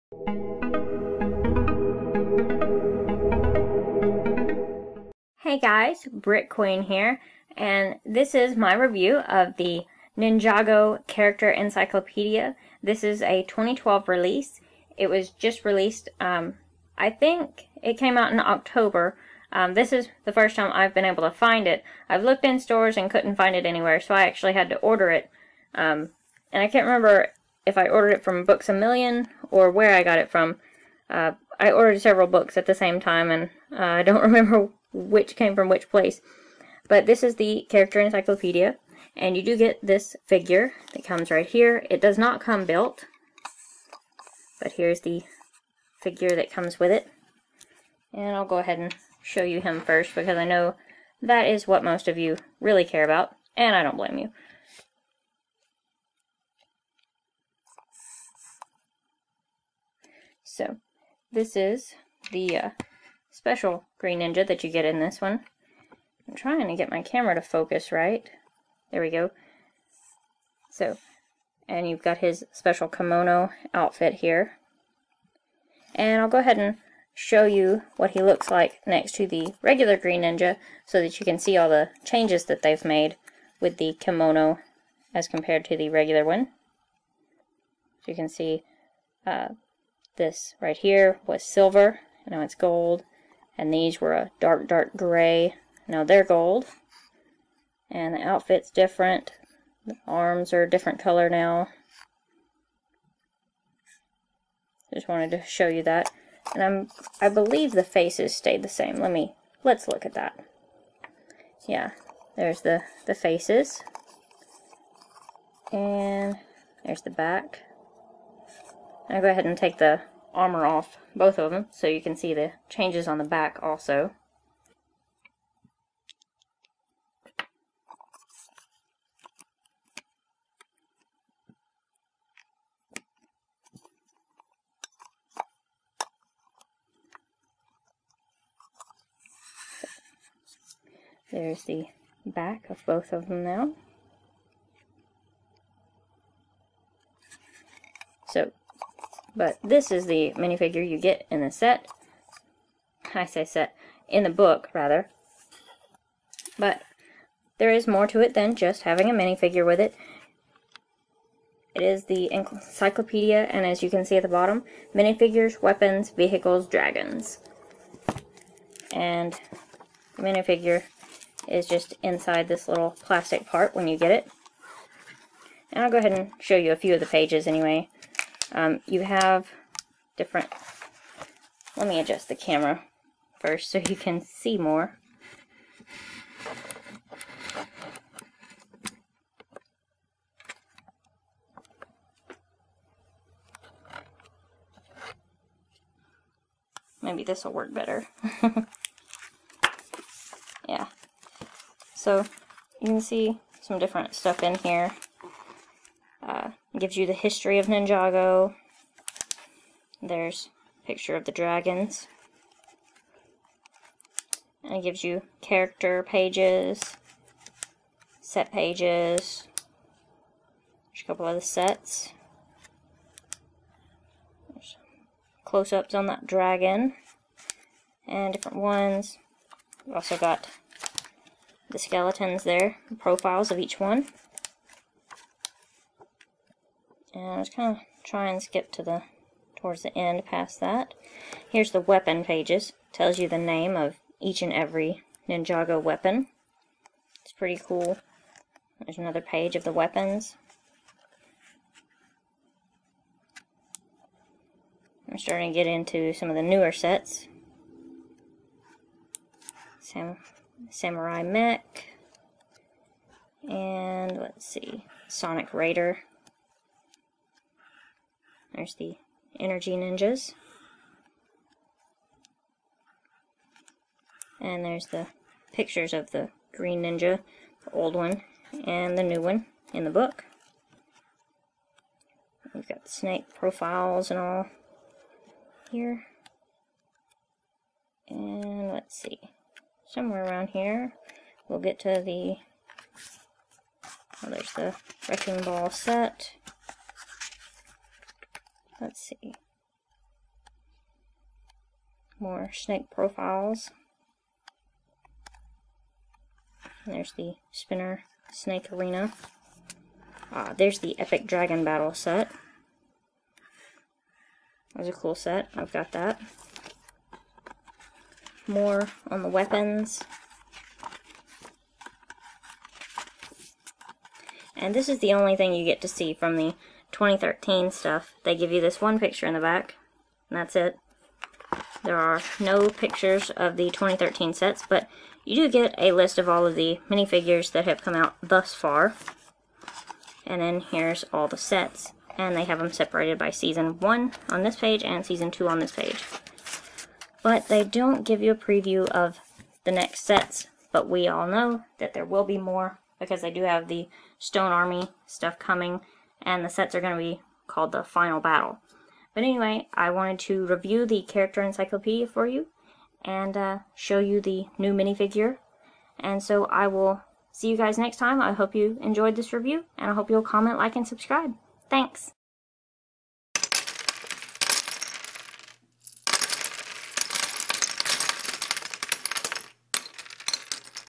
LEGO Ninjago Character Encyclopedia LEGO Book with Green Ninja Minifigure Review - BrickQueen
2012 LEGO Ninjago Character Encyclopedia and Lloyd Green Ninja with Kimono Minifigure Review. LEGO minifigure included is the exclusive Green Ninja with a Kimono.  This LEGO book was released in October 2012 and retails for between $15 and $20 depending on where you find it.

Please don't forget my other pages!!